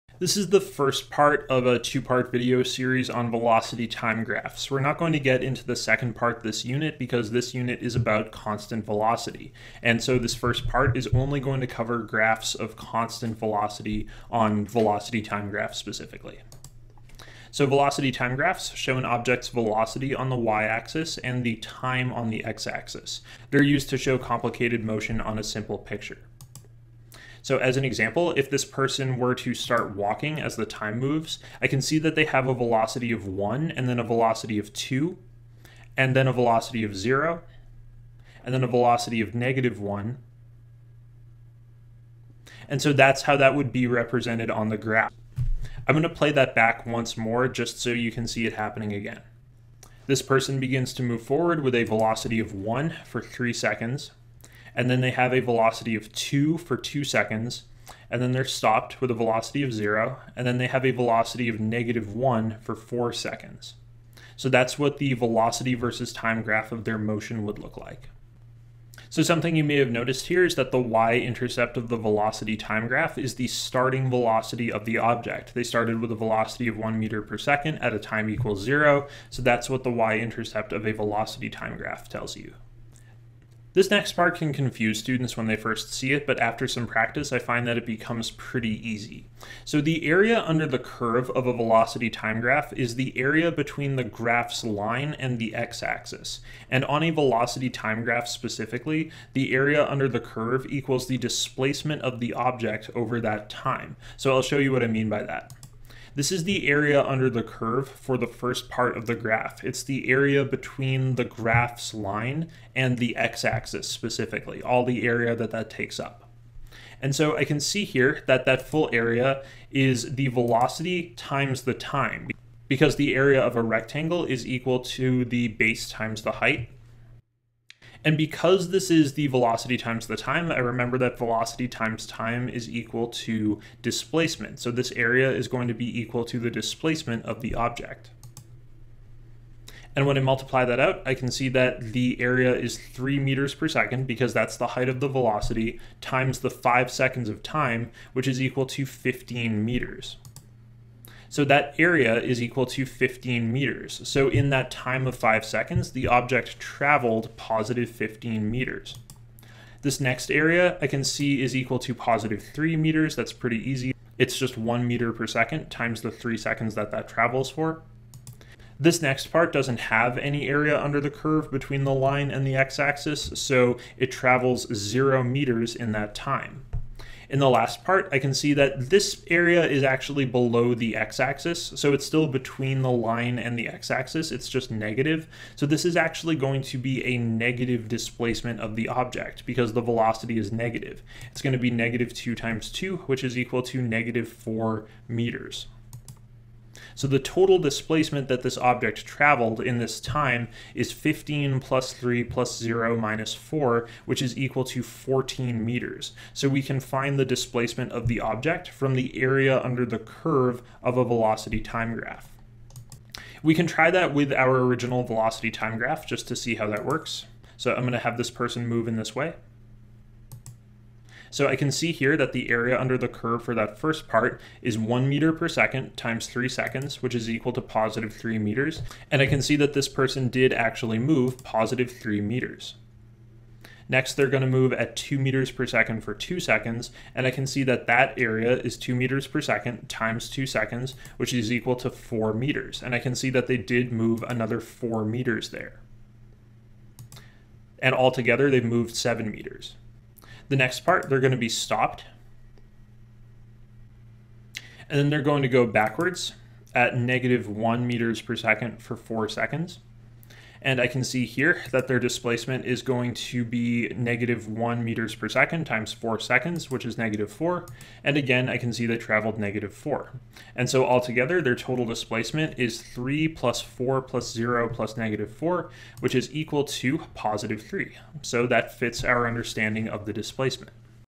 Velocity-Time Graphs: Constant Velocity and the Area Under the Curve - IB Physics
This video explains how to draw velocity time graphs for objects that have constant velocities, and goes over the properties of the graph. I show why the y-intercept of the graph is the initial velocity of the object and why the area under the curve of the graph is the displacement of the object.

0:00 Definition of velocity-time graph
0:26 Example velocity-time graph
1:08 Y-Intercept
1:23 Area under the curve
3:33 Example of area under the curve